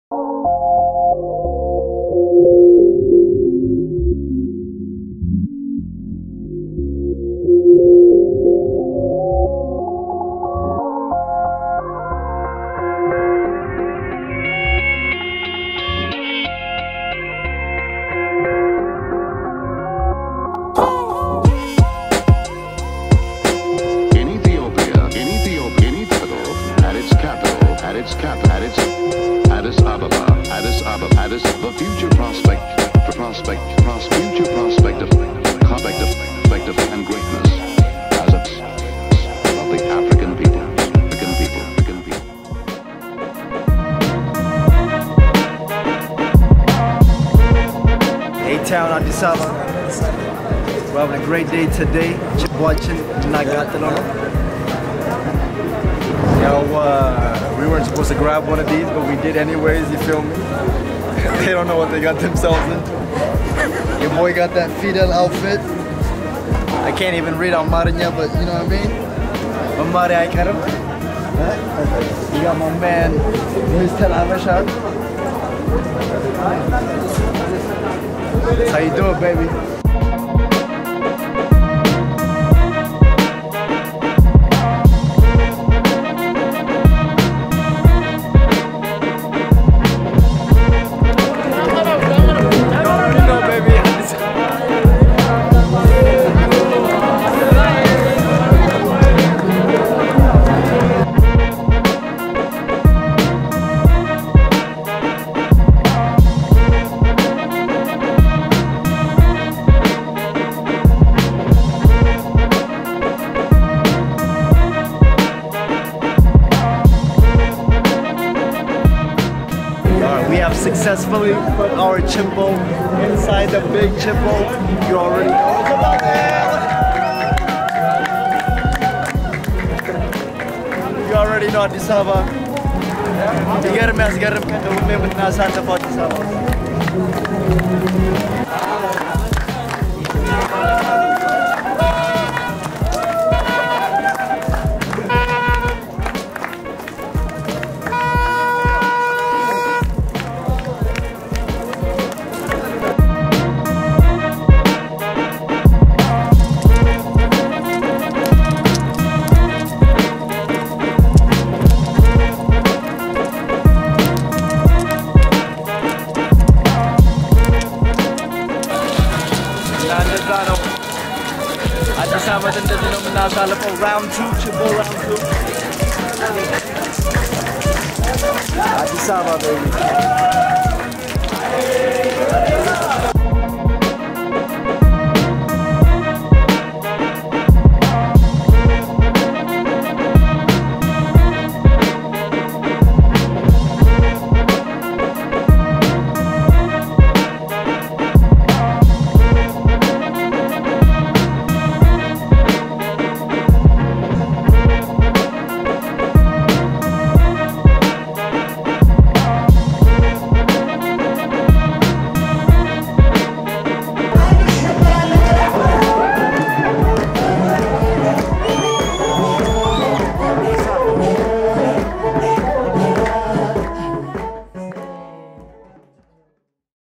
Meskel 2020 in Ethiopia - Special Episode
Meskel 2020 in Ethiopia!

Today's the holiday called MESKEL in Ethiopia!

Ethiopians are celebrating the annual Meskel festival, the first big festival of the Ethiopian religious year. According to Ethiopian Orthodox Christian tradition, the national holiday marks the finding of the cross that Jesus was crucified on.

We were able to go ahead and spend time with the homies at the traditional Ethiopian restaurant called Yod Abysiniya. Thank you for your time and efforts friends.